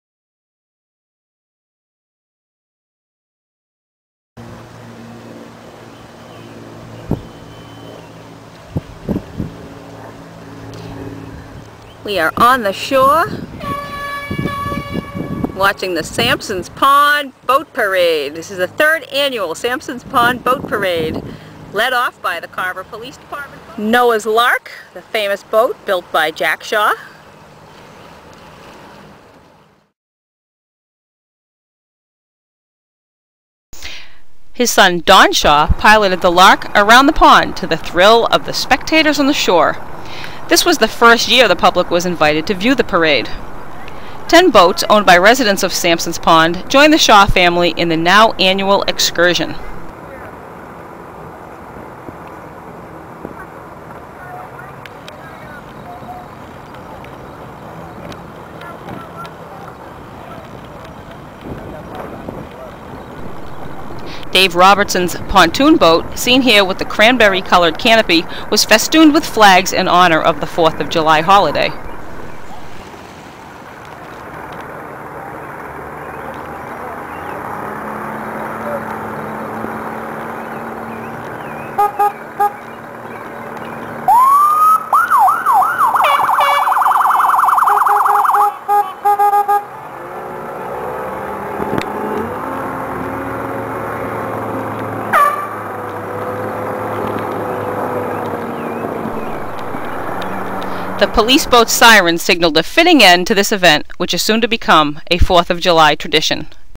3rd Annual Boat Parade
A parade of boats in Carver. Shot by Bobbi Sistrunk a journalist for the Carver Reporter.